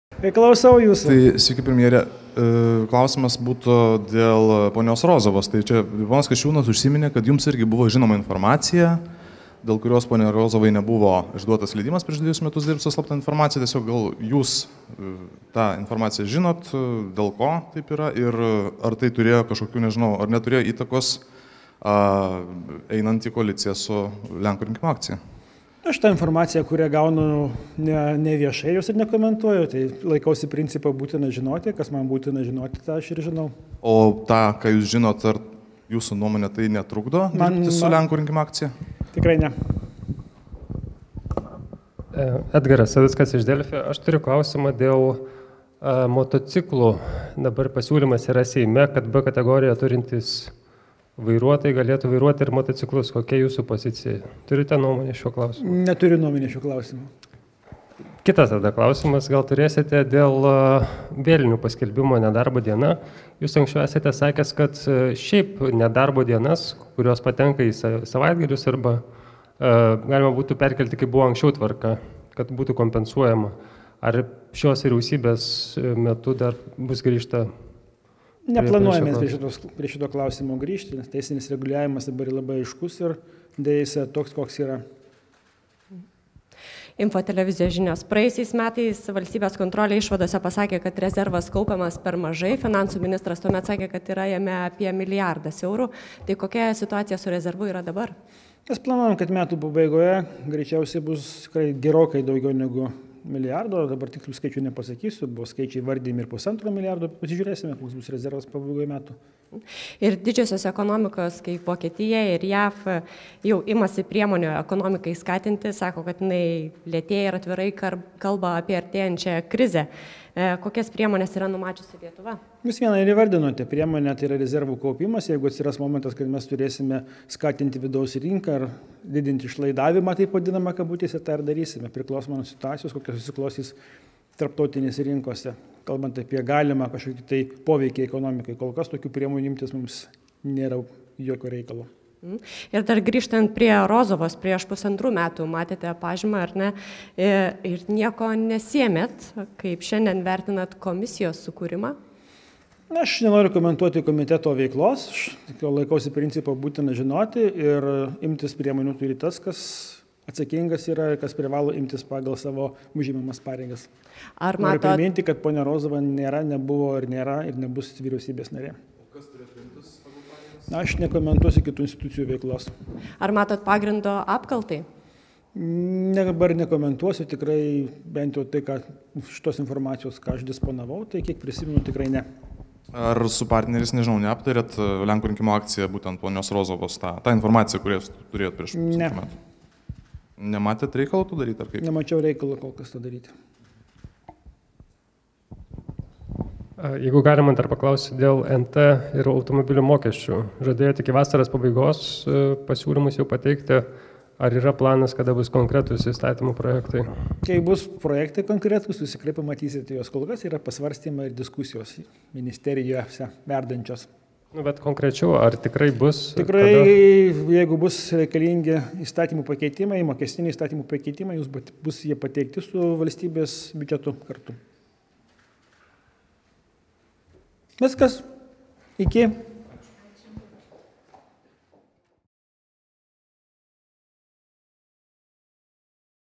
Premjeras S Skvernelis dalyvavo spaudos konferencijoje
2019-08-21 Premjeras S. Skvernelis dalyvavo spaudos konferencijoje po Vyriausybės posėdžio ir pasitarimo